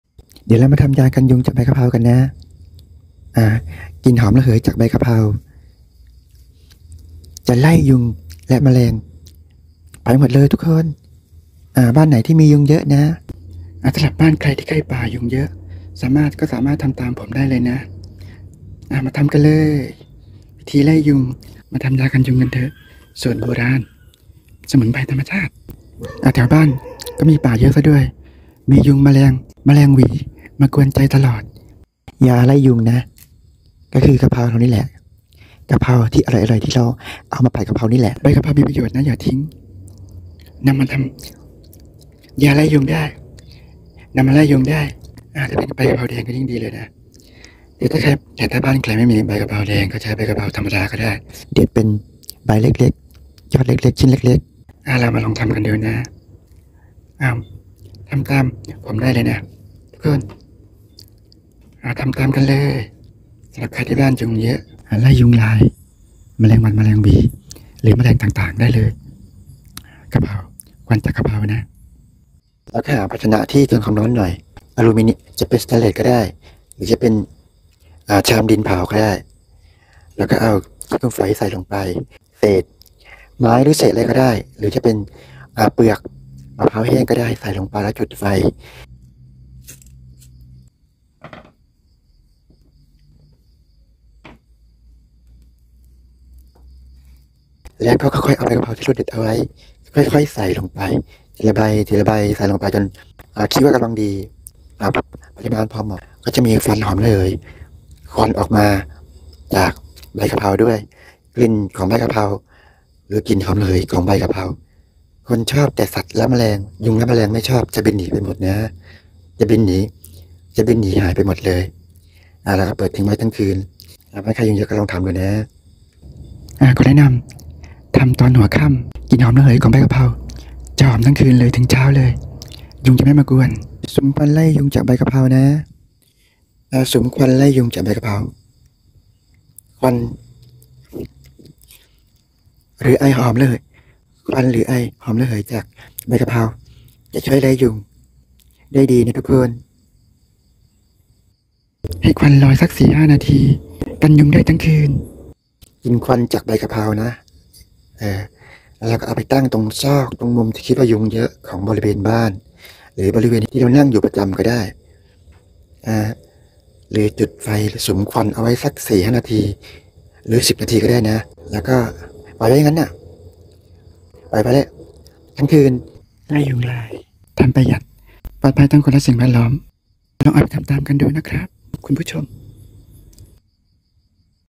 วิธีไล่ยุงด้วยสมุนไพรใบกระเพรา ประหยัด
บ้านใครที่อยู่ติดป่ามียุงเเละเเมลงต่างๆมากวนใจ เราสามารถใช้ใบกระเพราเเดง หรือกระเพราเขียวก็ได้มาทำป็นยาไล่ยุงเเมลงวัน เเมงหวี่ ได้
ไม่ใช้สารเคมีปลอดภัย เพราะใช้วัสดุจากธรรมชาติ
สุมควันไล่ยุง ยากันยุงลายจากสมุนไพรใบกระเพรา ประหยัด
ยุงลายหน้าฝน
ใบกระเพรามีประโยชน์หลายอย่าง